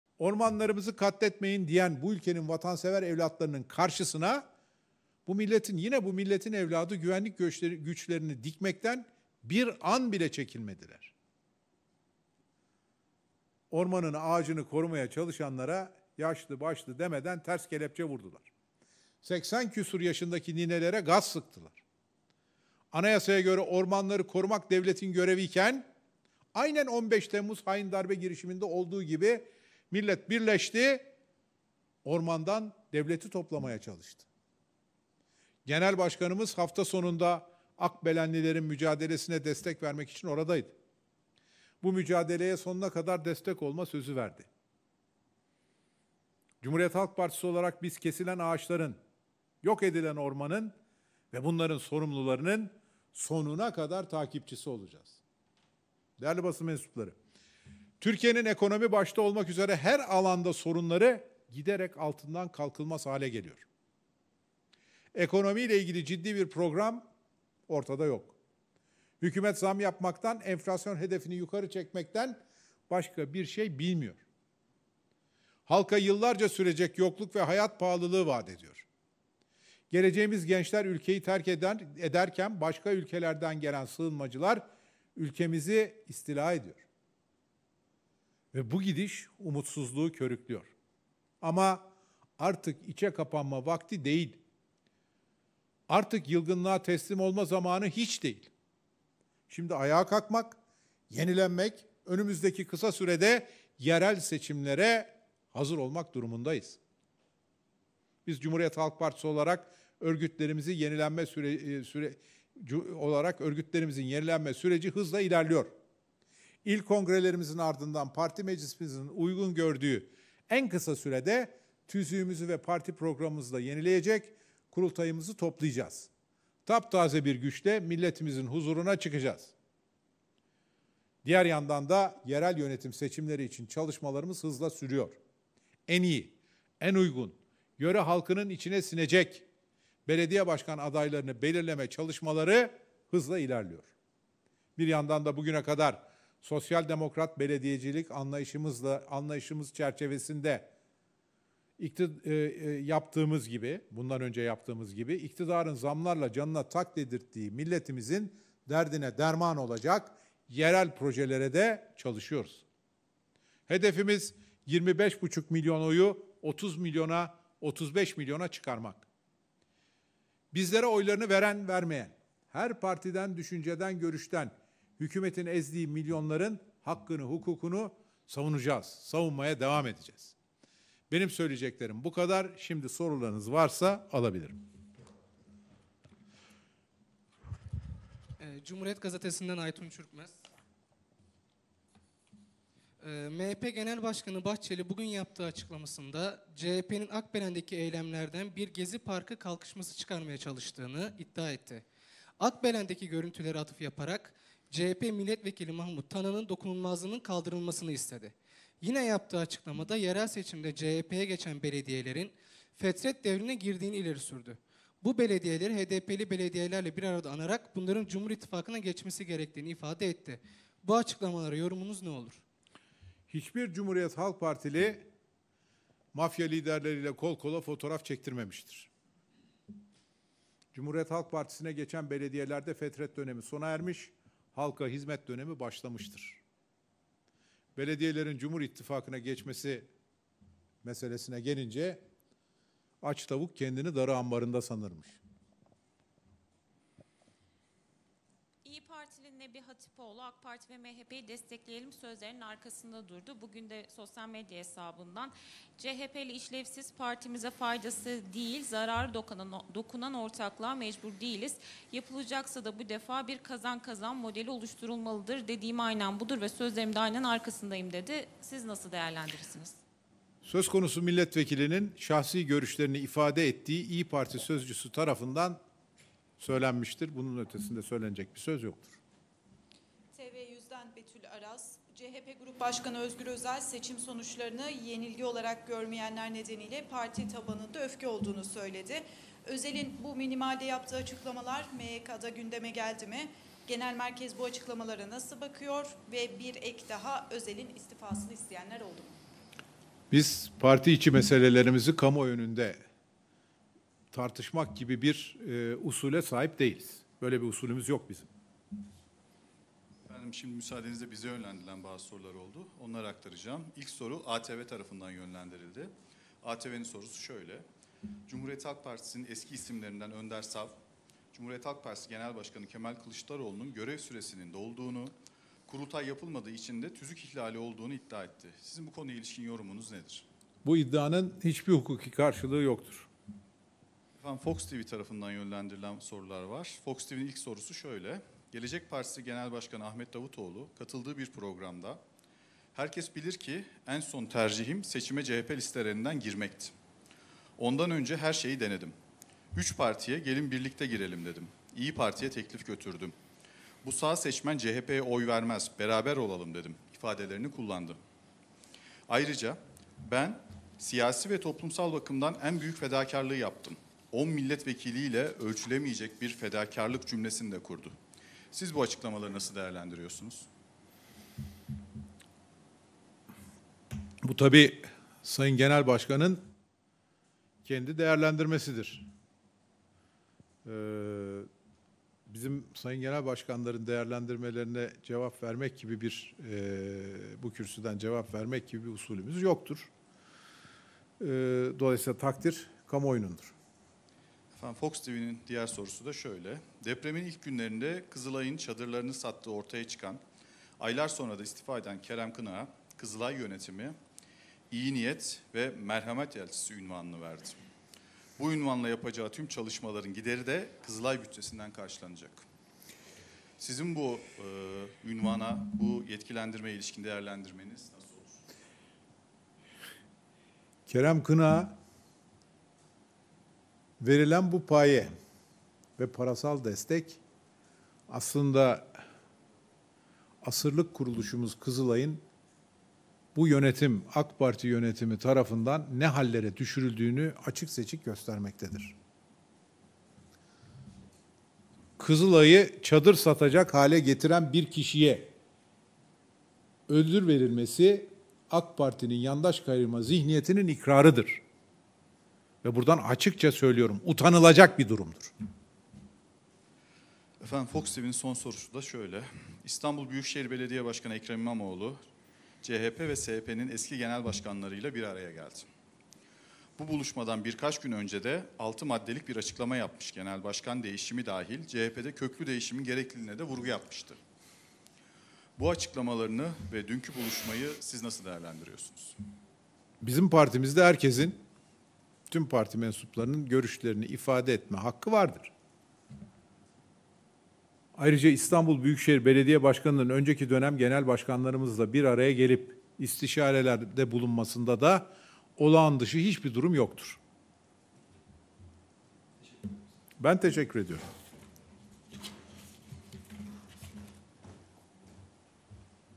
Davutoğlu, "en son tercihim seçime CHP listelerinden girmekti" demişti! CHP'den o sözlere ilk yanıt!
Cumhuriyet Halk Partisi Sözcüsü Faik Öztrak, Türkiye Büyük Millet Meclisi'nde düzenlediği basın toplantısında Gelecek Partisi Genel Başkanı Ahmet Davutoğlu'nun seçimlerle ilgili sözlerine yanıt verdi.